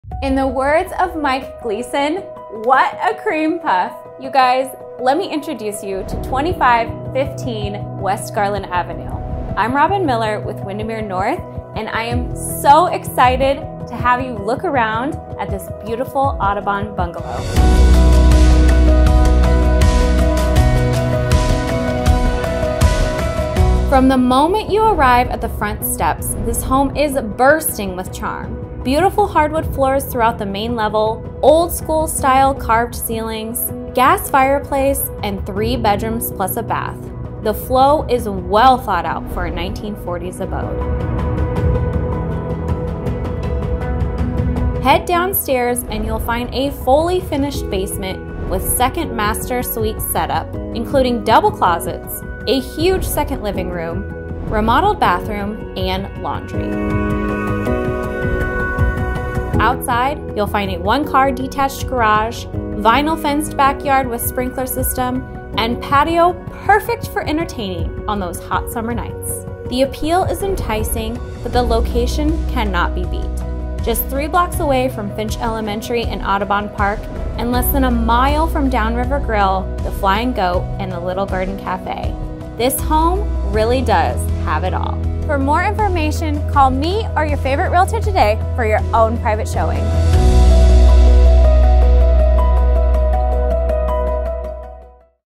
✨ 2515 W. Garland Avenue 🏡
This Audubon bungalow is such a cream puff 😍 Featuring 4 bedrooms, 2 bathrooms, nearly 2,300sq.ft. & just blocks away from Finch Elementary and Audubon Park ~ this charmer has it all!